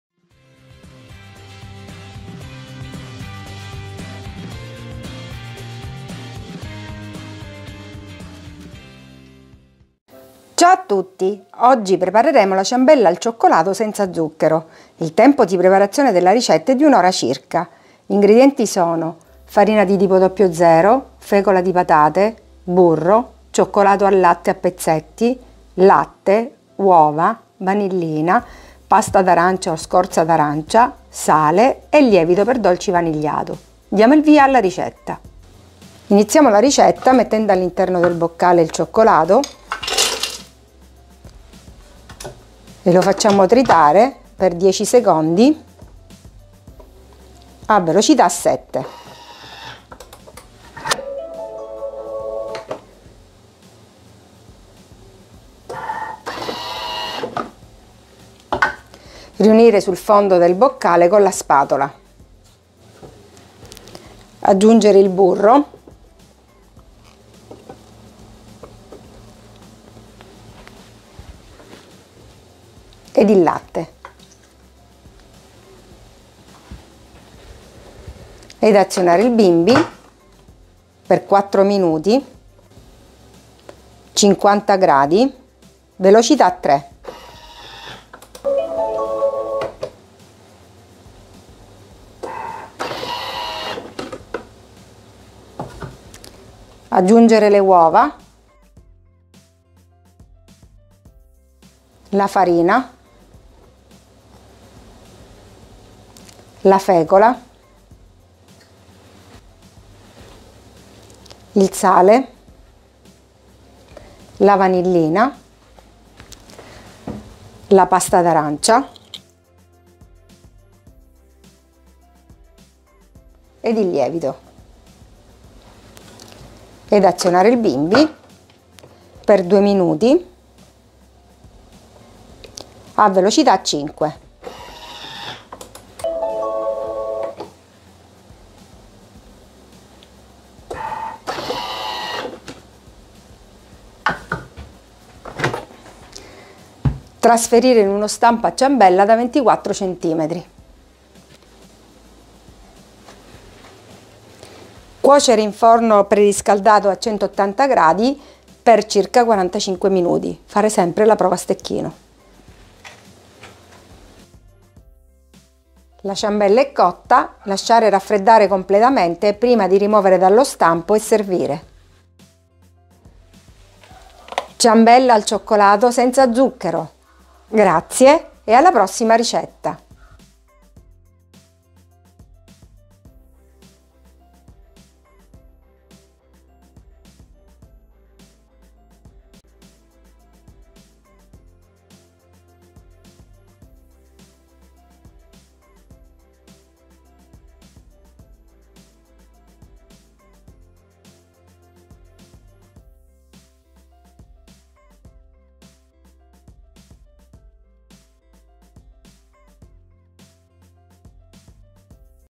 Ciambella al cioccolato senza zucchero per bimby TM6 TM5 TM31 TM21 riciclo uova Pasqua
Ingredienti:
150 g di farina di tipo "00"
50 g di fecola di patate
120 g di burro morbido
250 g di cioccolato al latte a pezzetti
100 g di latte
4 uova
1 bustina di vanillina
1 cucchiaino di pasta d'arancia o scorza di un arancia
1 pizzico di sale
1 bustina di lievito per dolci vanigliato